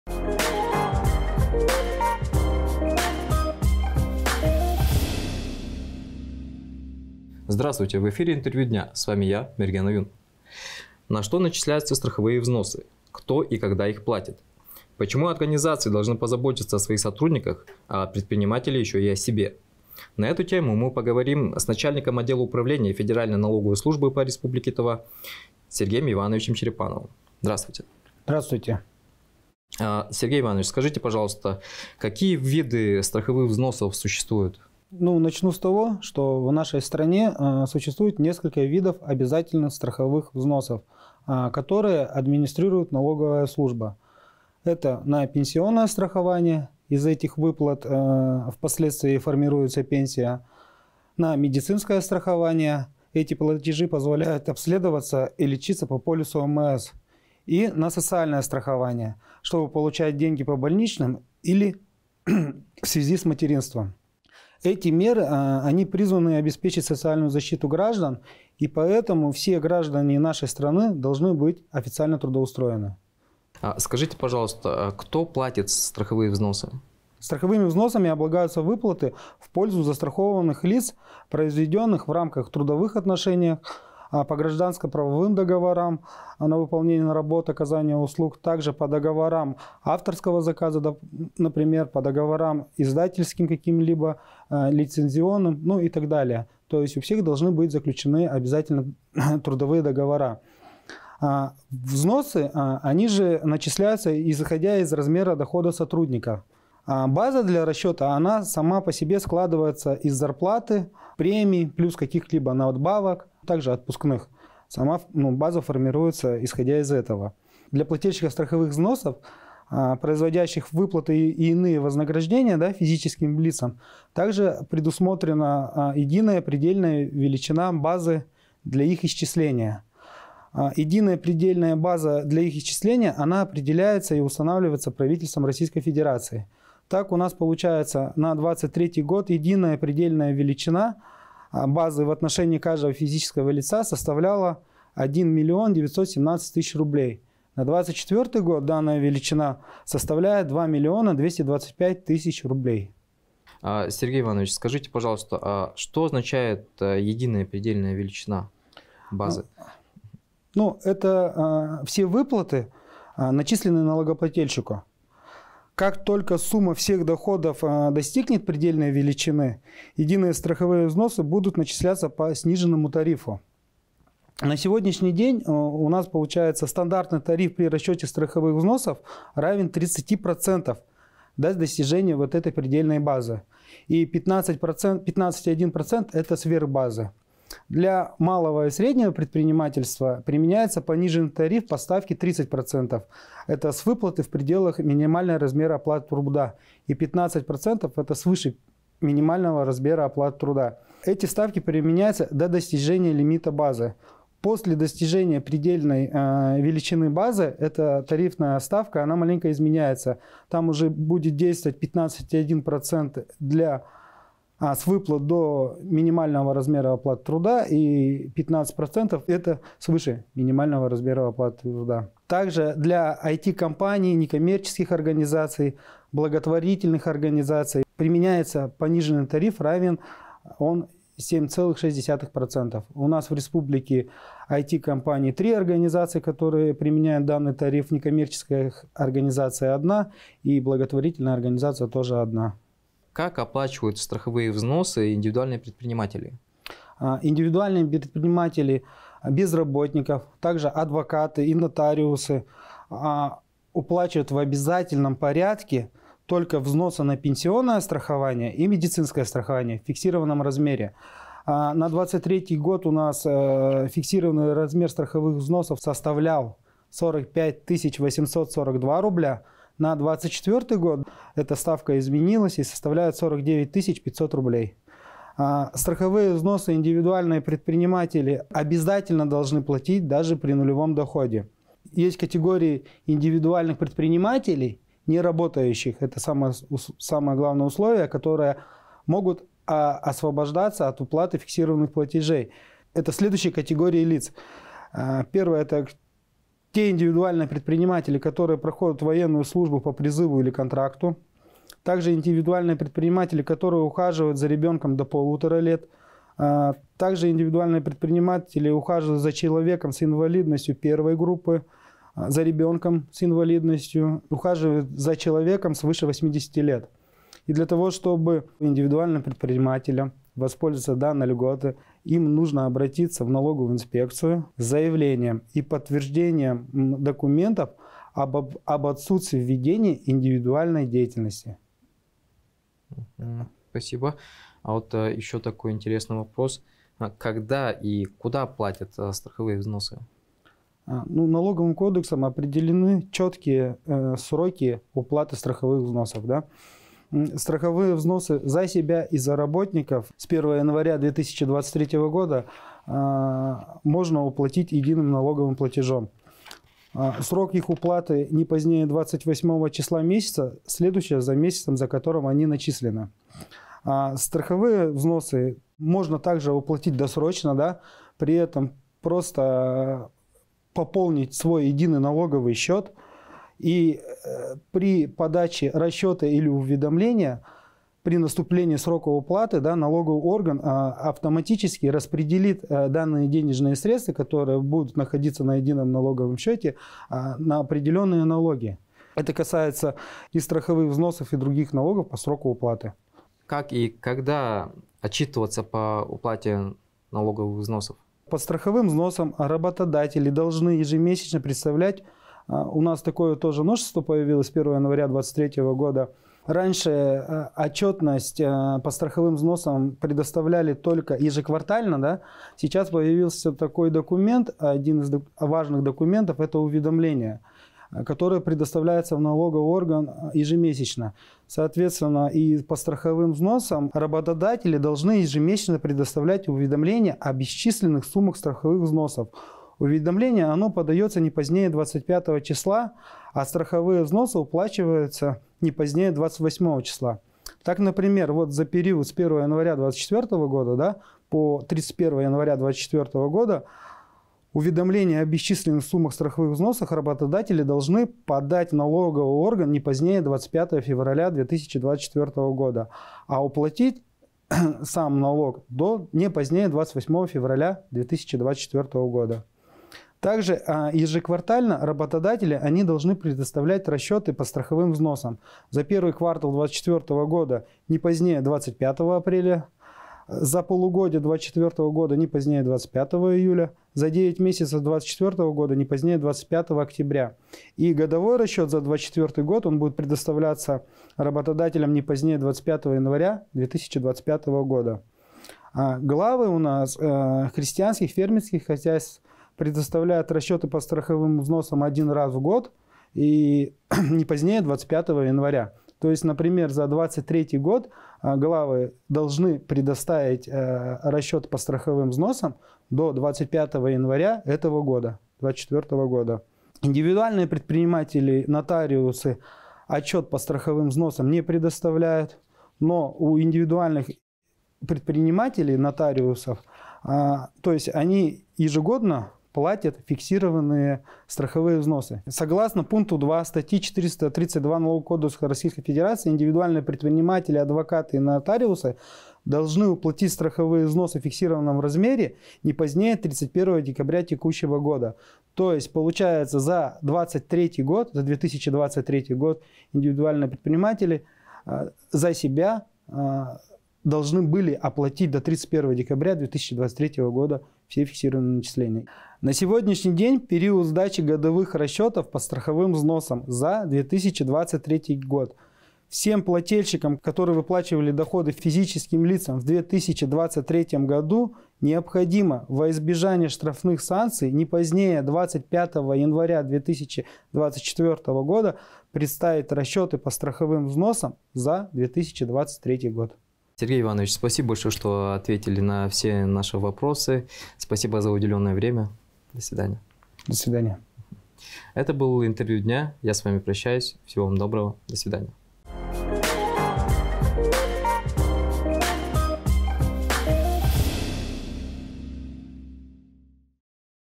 На что начисляются страховые взносы, кто и когда их платит?
На что начисляются страховые взносы, кто и когда их платит. Почему организации должны позаботиться о своих сотрудниках, а предприниматели еще и о себе. На эту тему поговорим с начальником отдела Управления Федеральной налоговой службы по РТ Сергеем Черепановым.